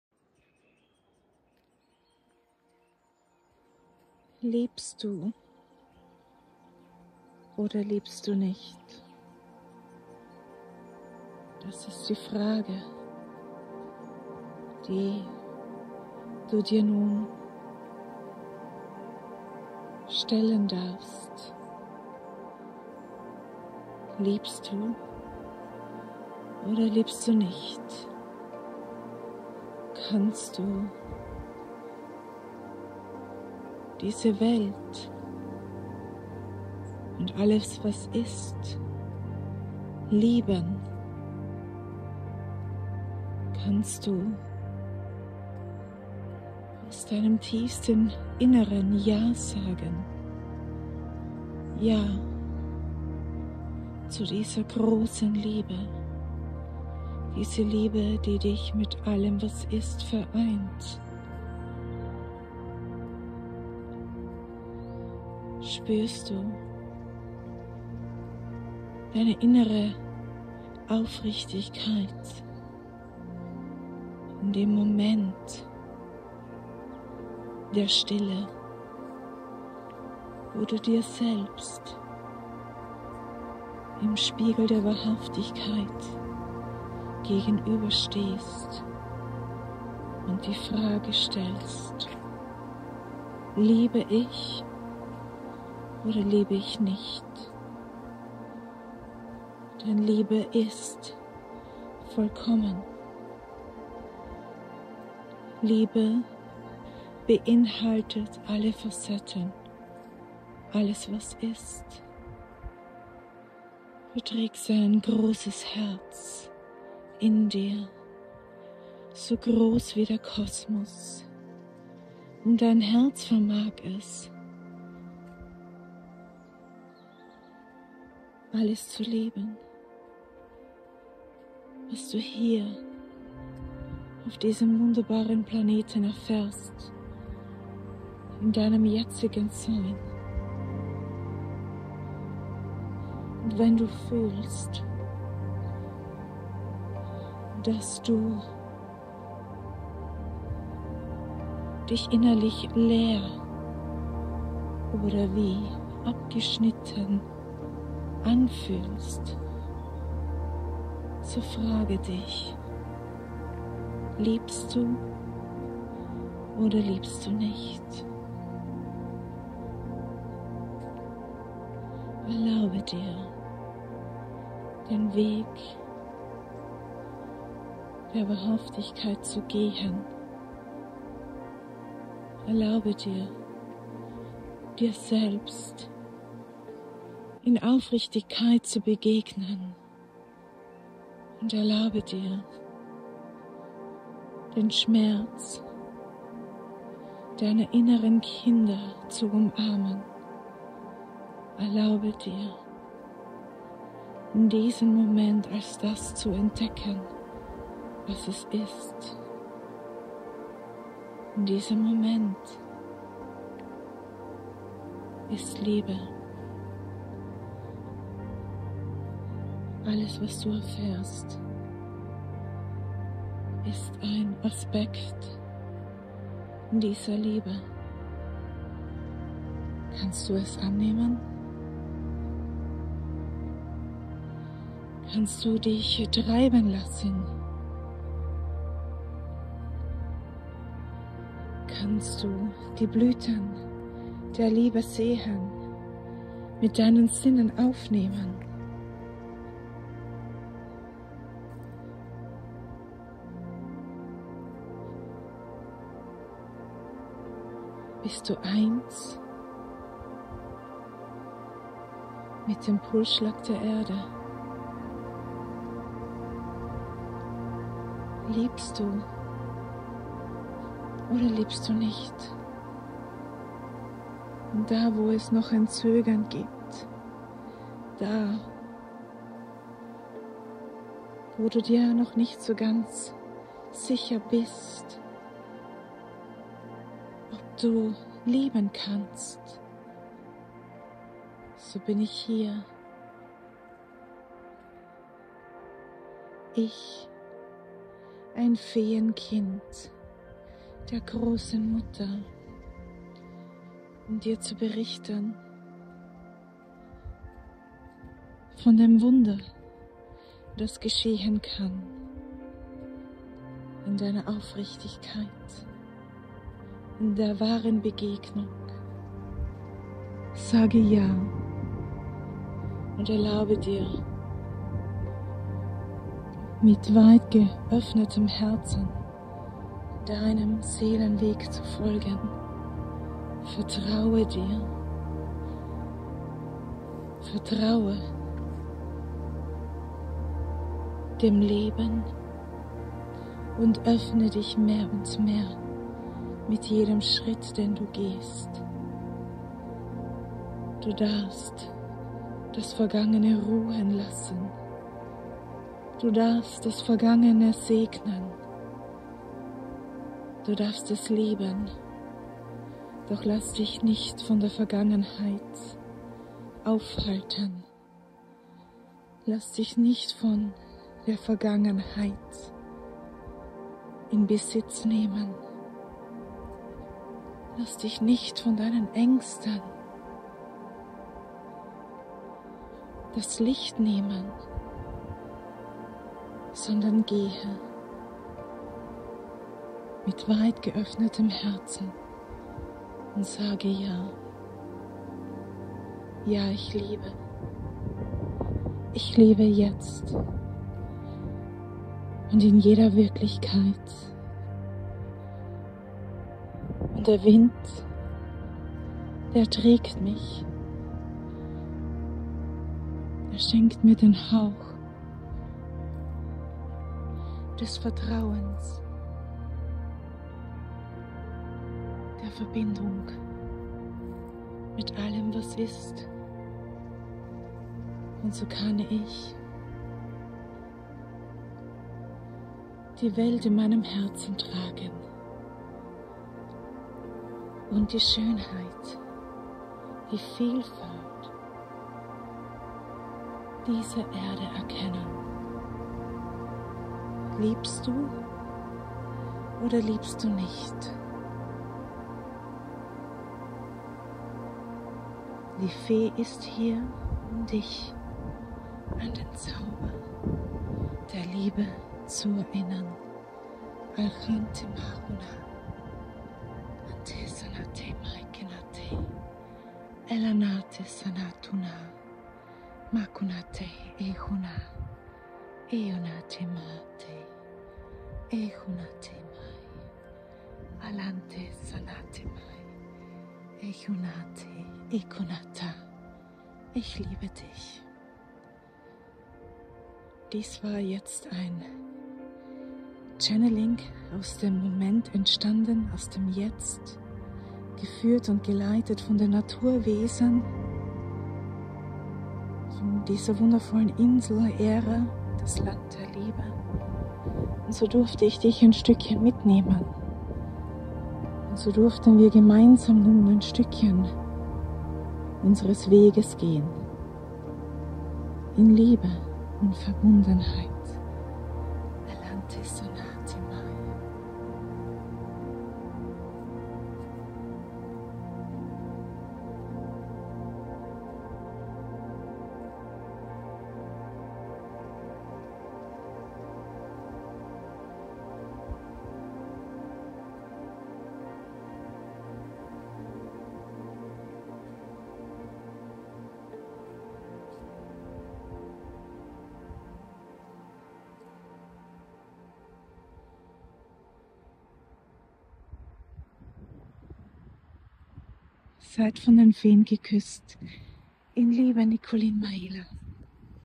Liebst du? Oder liebst du nicht? Channeling aus dem Jetzt
Tagesimpuls 💎 Channeling aus dem Jetzt 💎

Ich nehme dich mit auf meinem Spaziergang und schenke dir die Tagesbotschaft der Fee 🧚🏻

Liebst du? Oder liebst du nicht?
￼Fühle die Liebesessenz der Fee 🙏🏻🧚🏻🌱

Möchtest du bei dem kommenden ONLINE IRLAND RETREAT teilnehmen?

♥️ Dankbarkeit🙏🏻✨

Ich danke dir für deine Wertschätzung, damit unterstützt du mich und diesen Kanal. 🫶
Möge es in Liebe zu dir zurückkehren.